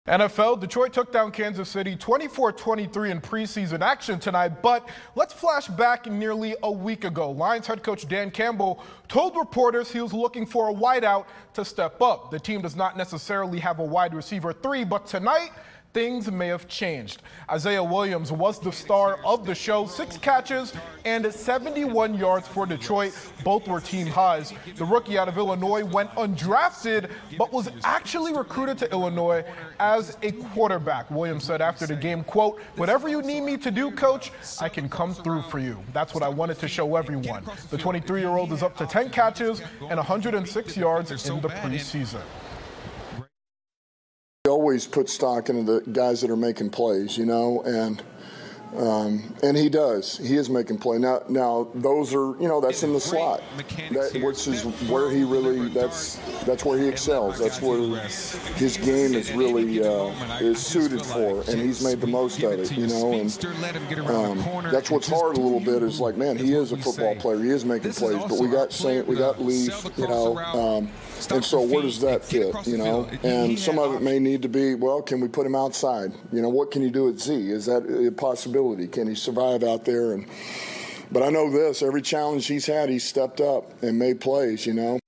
Isaiah Williams makes case for Lions' WR3 | 13 ON YOUR SIDE
Williams tallied 6 catches and 71 yards for Detroit in its second pre-season game against Kansas City.